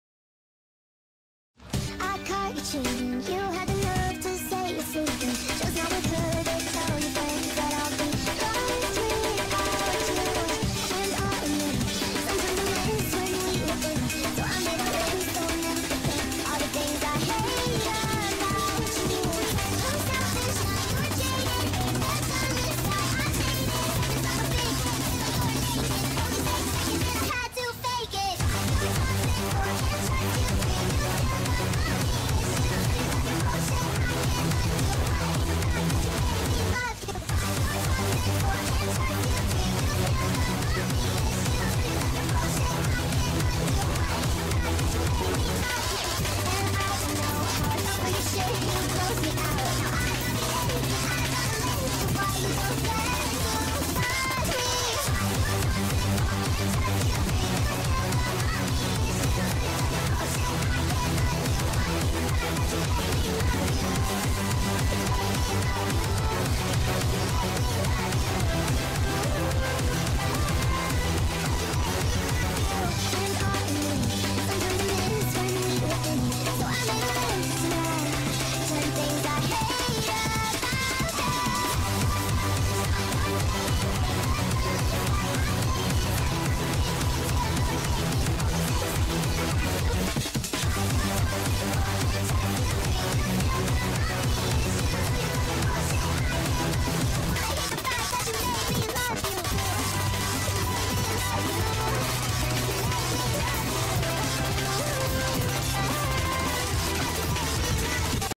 This is dumb (Obviously sped up)
I played the map at 30% speed. I'm not a challenge player lol